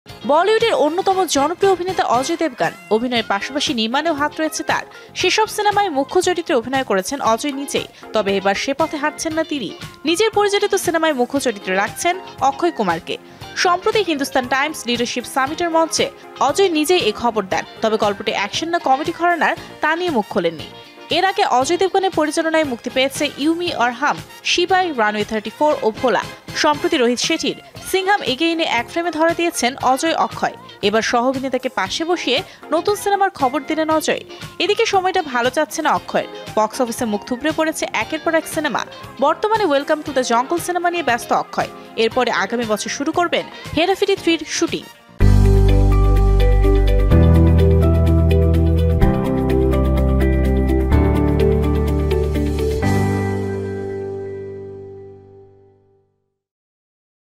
আসছে অজয় দেবগন পরিচালিত পঞ্চম সিনেমা, নায়ক অক্ষয় কুমার | Ajay Devgn | Akshay Kumar | Jamuna TV
বলিউডের অন্যতম জনপ্রিয় অভিনেতা অজয় দেবগন। অভিনয়ের পাশাপাশি নির্মাণেও হাত রয়েছে তার। সেসব সিনেমায় মুখ্য চরিত্রে অভিনয় করেছেন অজয় নিজেই। তবে এবার সে পথে হাঁটছেন না তিনি। নিজের পরিচালিত সিনেমায় মুখ্য চরিত্রে রাখছেন অক্ষয় কুমারকে। সম্প্রতি হিন্দুস্থান টাইমস লিডারশিপ সামিটের মঞ্চে অজয় নিজেই এই খবর দেন